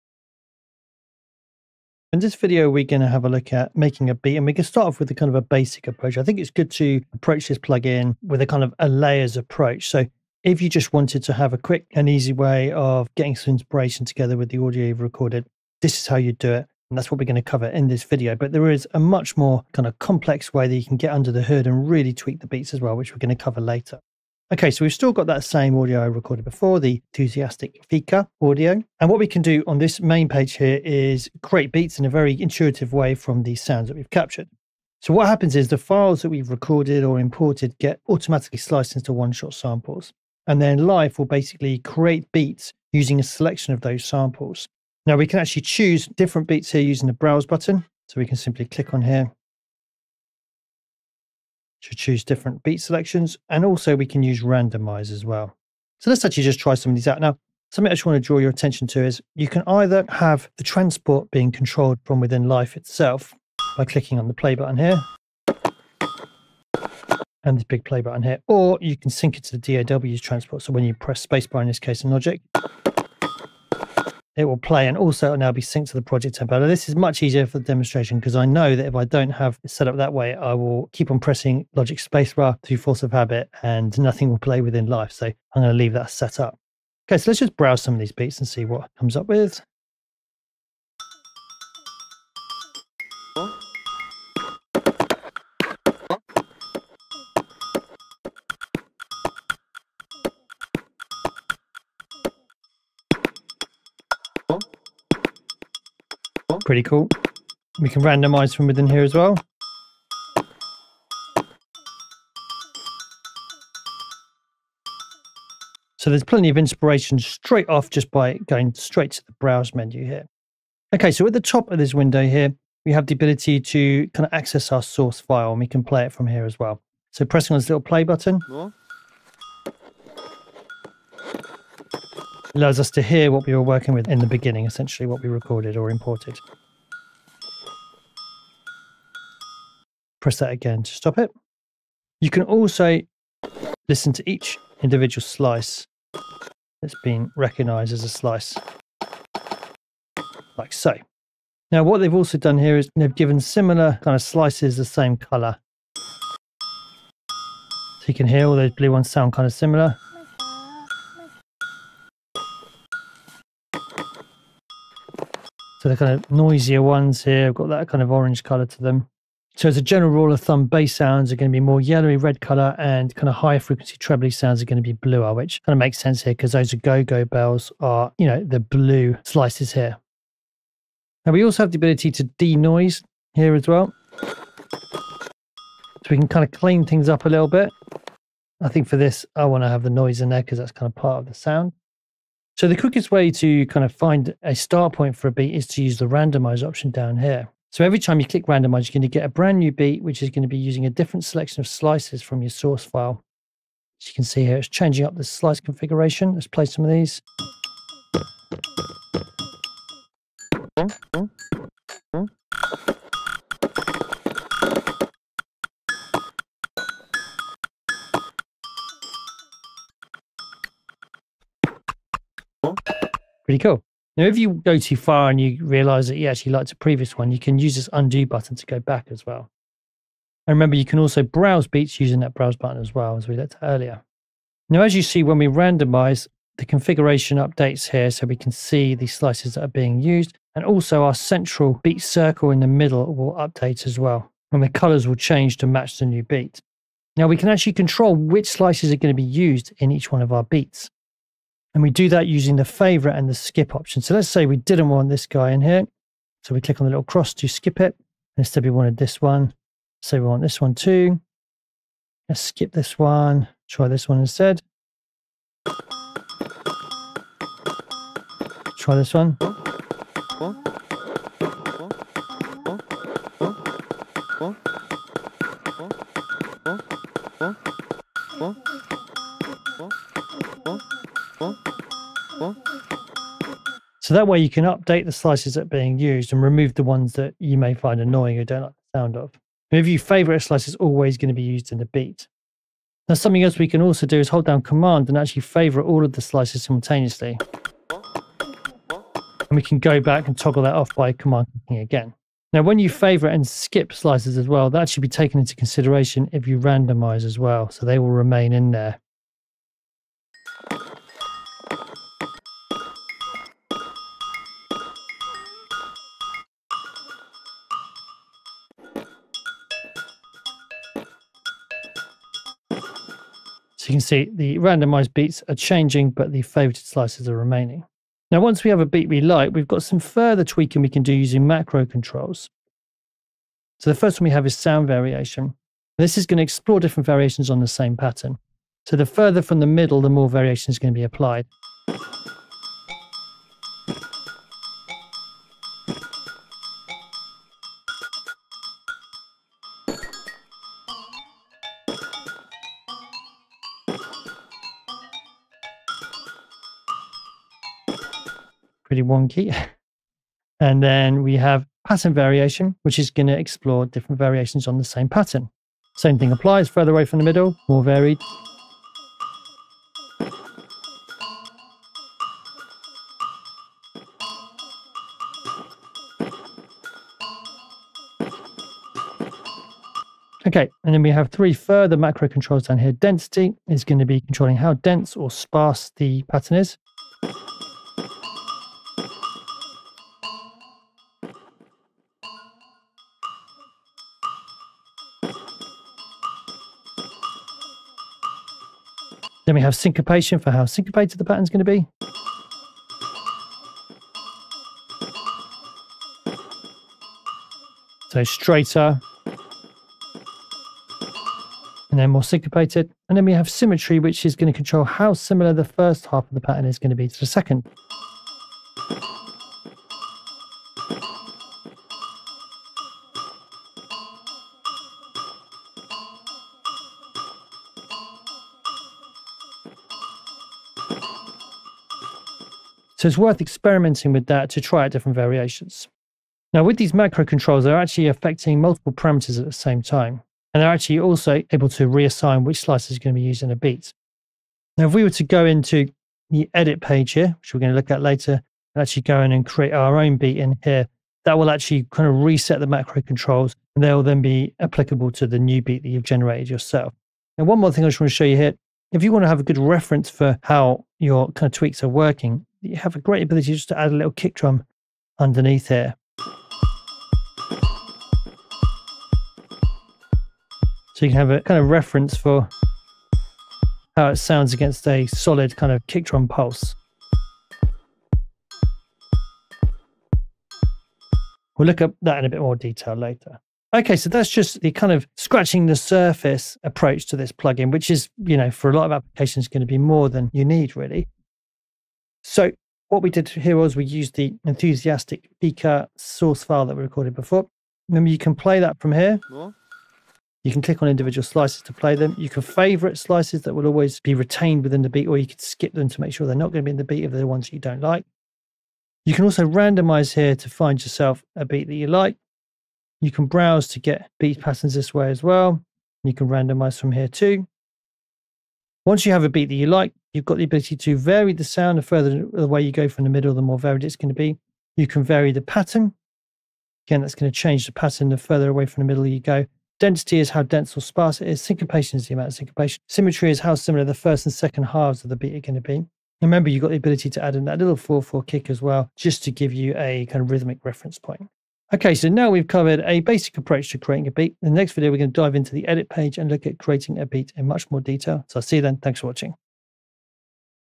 XLN Audio Life: Making a Beat Using the Explore Page
Learn how to create a beat in the Explore page. This relies mostly on randomizing the sounds and patterns and then amending using the macro controls. It's a quick and intuitive way of getting great results fast.